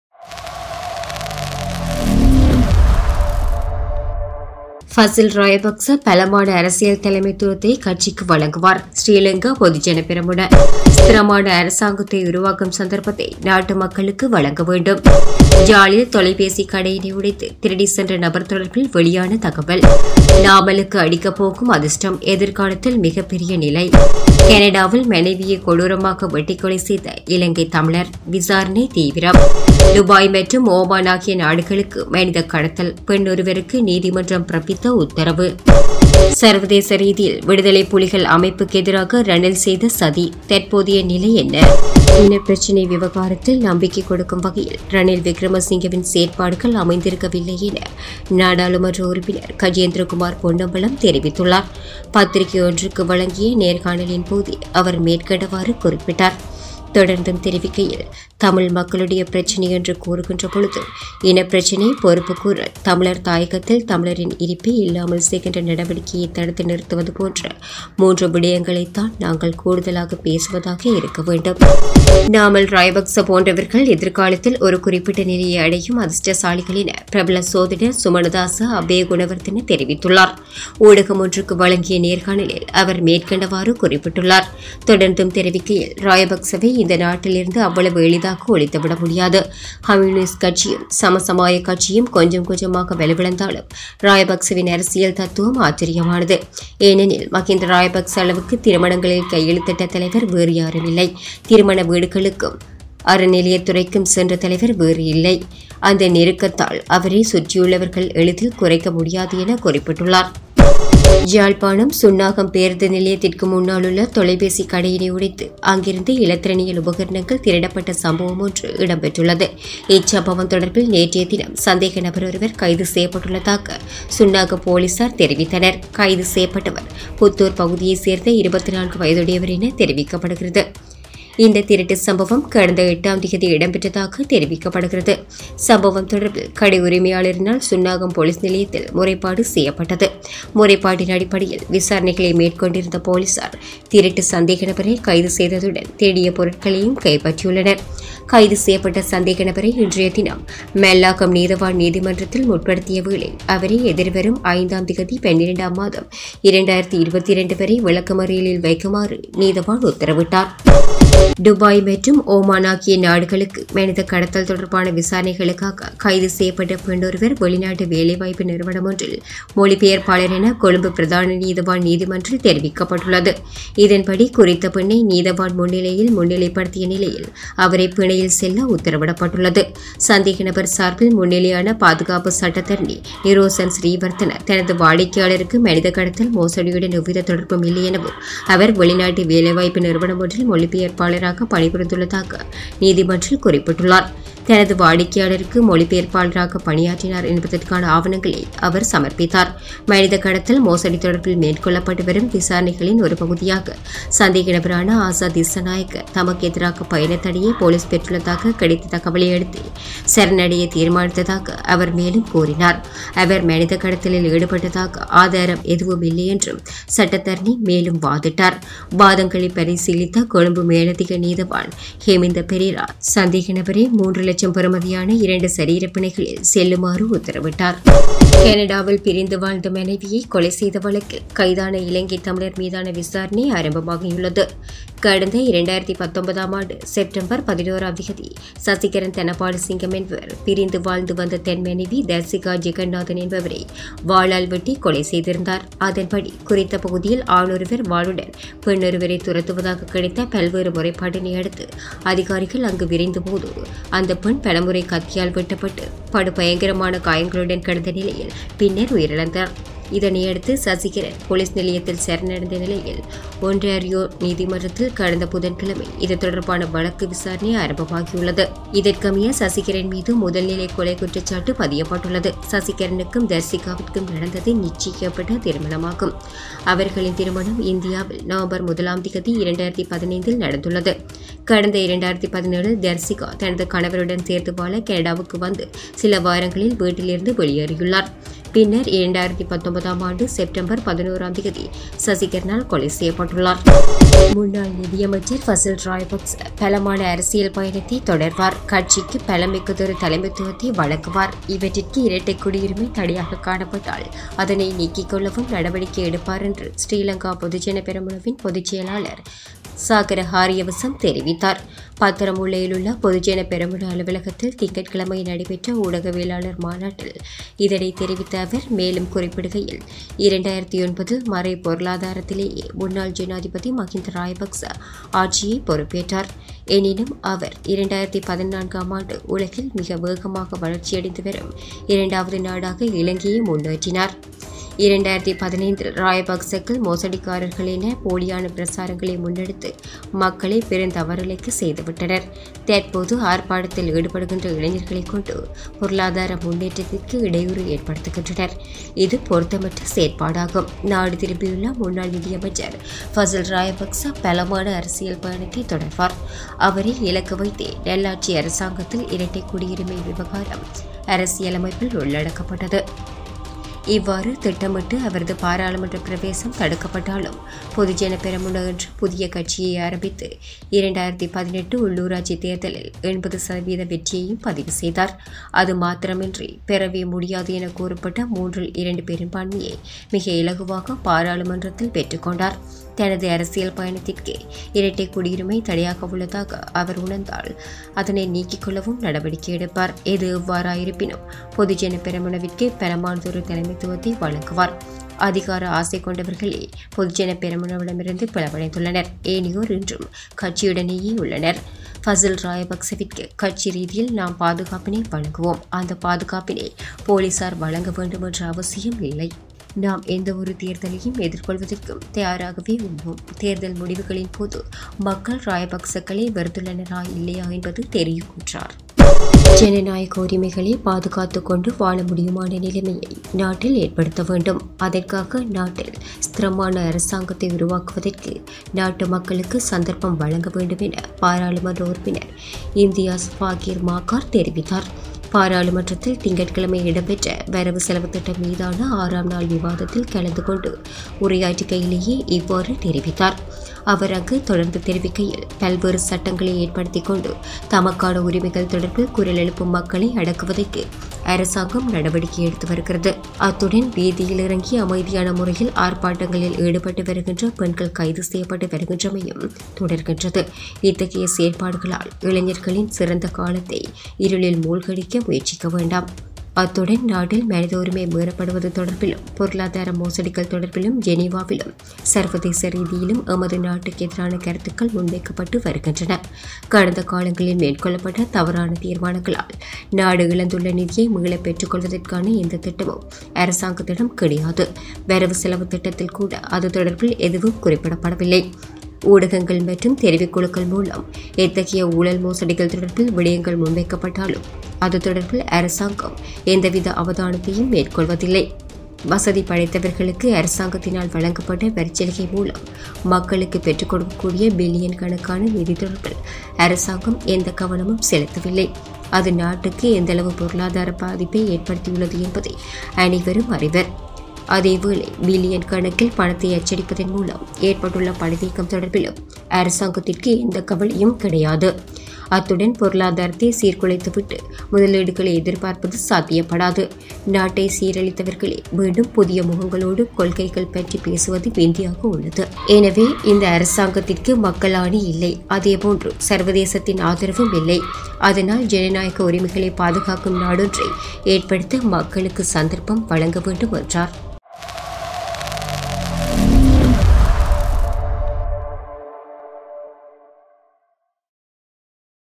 இலங்கையின் இன்றைய முக்கிய செய்திகள் | பிரதான செய்திகள் 22.11.2022 | Today Sri Lanka Tamil News
-சர்வதேச ரீதியில் விடுதலைப் புலிகள் அமைப்புக்கு எதிராக ரணில் செய்த சதி! தற்போதைய நிலை என்ன..
-பஷில் ராஜபக்ஷ பலமான அரசியல் தலைமைத்துவத்தை கட்சிக்கு வழங்குவார் - ஸ்ரீலங்கா பொதுஜன பெரமுன
-ஸ்திரமான அரசாங்கத்தை உருவாக்கும் சந்தர்ப்பத்தை நாட்டு மக்களுக்கு வழங்க வேண்டும் - இம்தியாஸ் பாக்கீர் மாக்கார்
-யாழில் தொலைபேசி கடையினை உடைத்து திருடிச்சென்ற நபர் தொடர்பில் வெளியான தகவல்
-நாமலுக்கு அடிக்கப் போகும் அதிர்ஷ்டம்! எதிர்காலத்தில் மிகப்பெரிய நிலை
-கனடாவில் மனைவியை கொடூரமாக வெட்டிக்கொலை செய்த இலங்கை தமிழர்! விசாரணை தீவிரம்
-டுபாய் மற்றும் ஓமன் ஆகிய நாடுகளுக்கு மனித கடத்தல்! பெண் ஒருவருக்கு நீதிமன்றம் பிறப்பித்த உத்தரவு
-அரசாங்கம்  முன்னெடுக்கும் திட்டங்களுக்கு நாம் ஒருபோதும் ஆதரவளிக்க மாட்டோம் - கஜேந்திரகுமார்  பகிரங்க அறிவிப்பு
-இலங்கையில் 50 ஆயிரம்  சிறுவர்கள் ஊட்டச்சத்து குறைபாட்டினால் பாதிப்பு
- இரட்டைக் குடியுரிமையை நீக்கிக்கொள்வதற்கு பசில் ராஜபக்ஷ நடவடிக்கை எடுப்பார் – சாகர காரியவசம்
-வறிய குடும்பங்களை ஏமாற்றி சிறுநீரகம் விற்பனை-கொழும்பில் அதிர்ச்சி சம்பவம்
-வரவு செலவுத்திட்ட முன்மொழிவுக்கு எதிராக வாக்களிக்க ஐக்கிய மக்கள் சக்தி தீர்மானம்
-QR குறியீட்டு முறையை நீக்குவது தொடர்பில் தீர்மானம் எட்டப்படவில்லை – காஞ்சன விஜேசேகர
-இலங்கைப் பெண்கள் வெளிநாடுகளுக்கு விற்பனை - இலங்கைத் தூதரகத்தின் செயலாளர் மறுப்பு